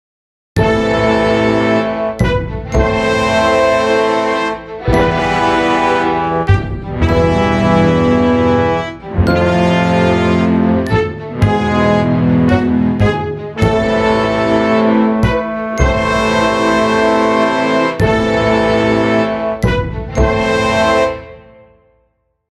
Make iPad your own orchestra/choir : my own music no name yet
Everything done by iPad Pro 2020/garageband/ iOS music production (RAW NOT EDITED YET) instagram